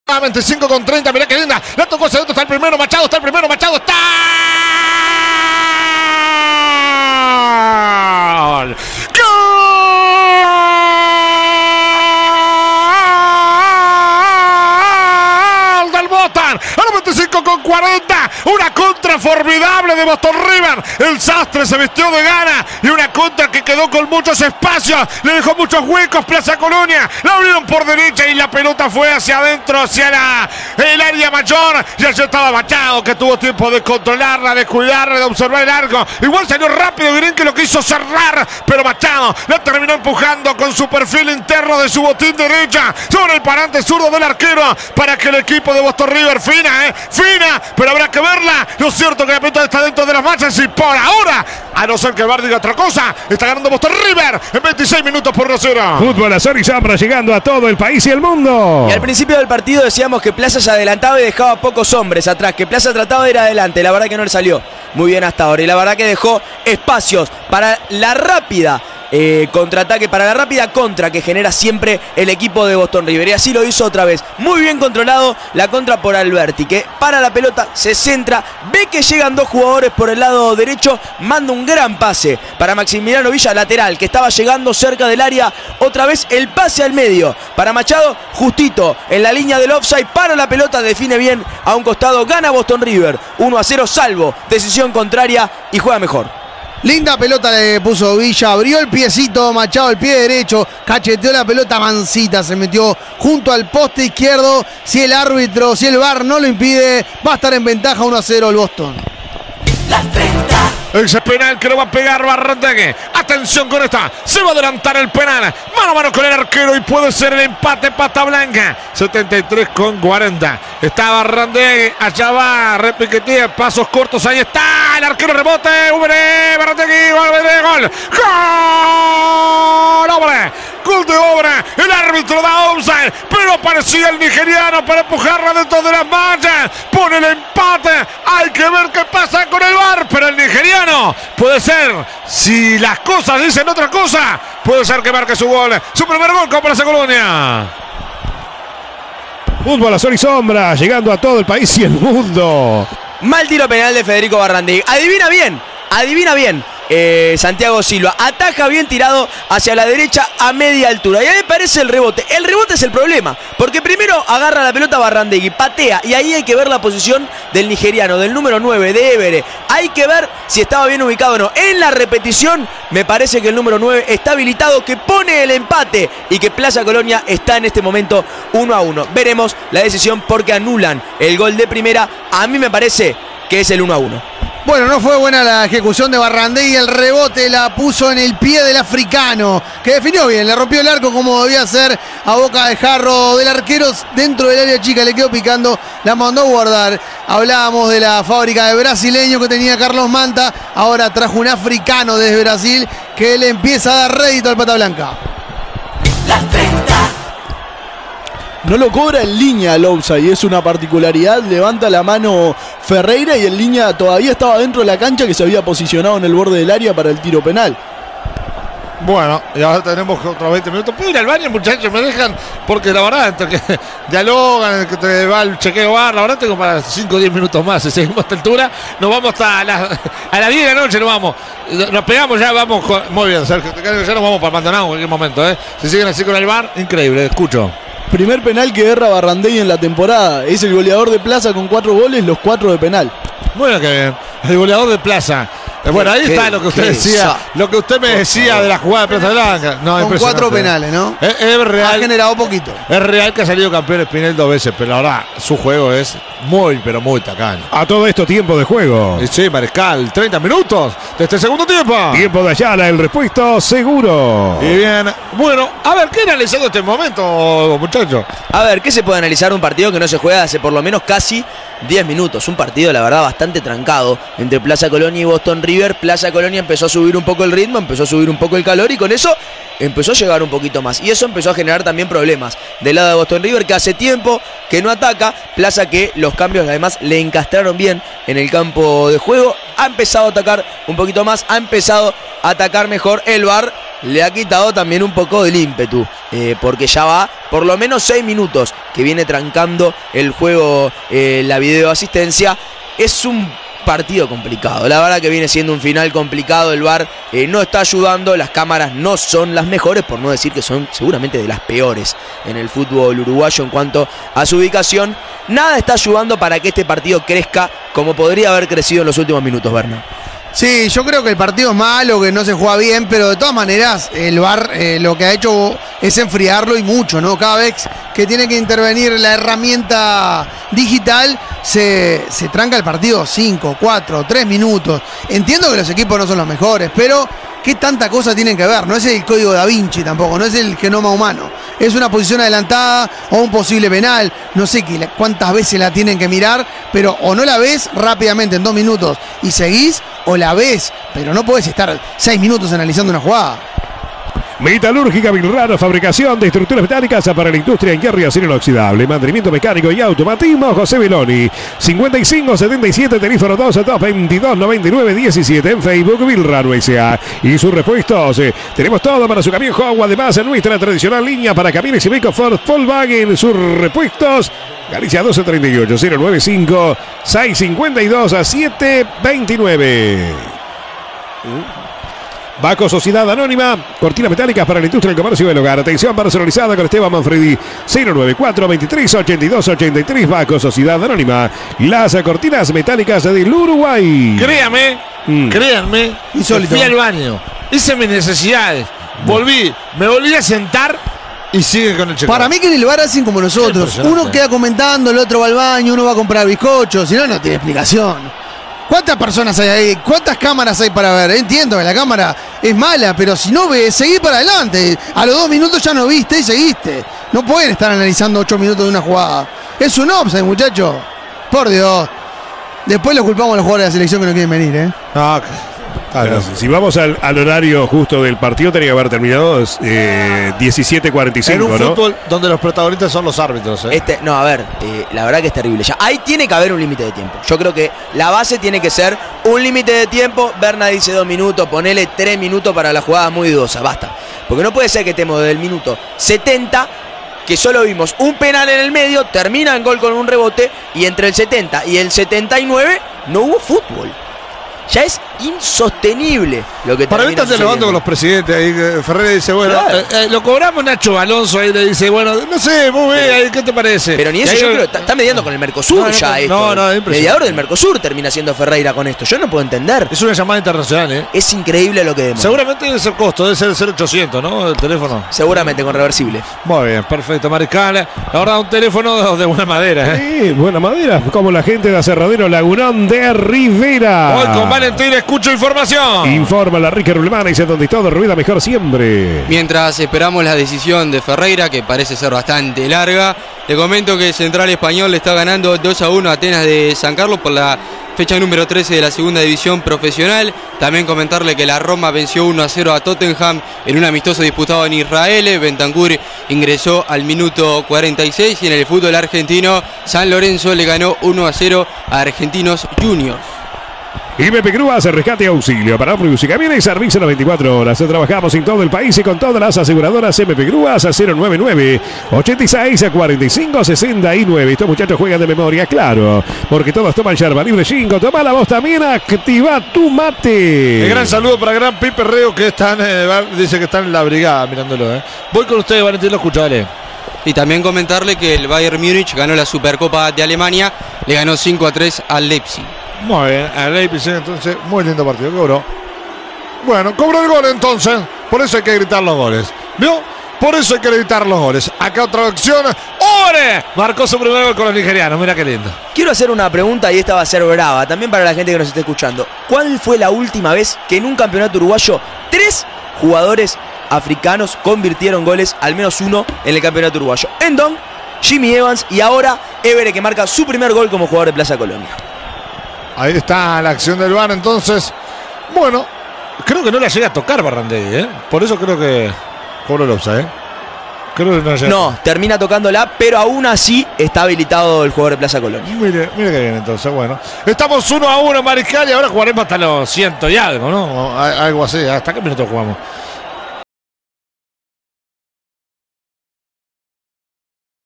Plaza Colonia 1 Boston River 1 - Clausura 2022 (por Gonzalo Fassanello)
Los goles del partido, por Gonzalo Fassanello para Radio Nacional